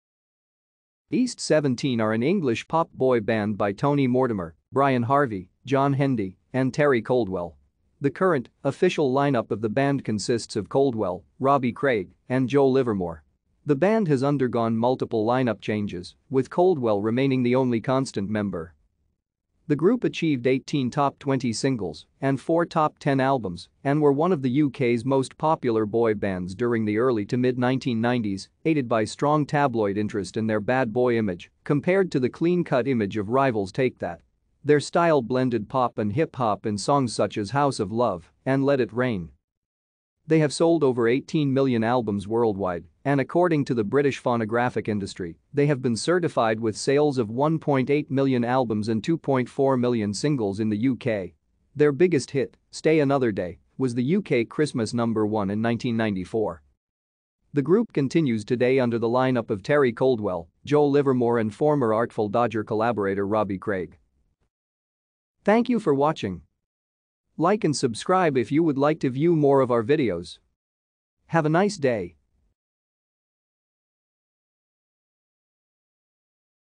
East 17
East 17 are an English pop boy band by Tony Mortimer, Brian Harvey, John Hendy, and Terry Coldwell. The current, official lineup of the band  consists of Coldwell, Robbie Craig, and Joe Livermore. The band has undergone multiple lineup changes, with Coldwell remaining the only constant member.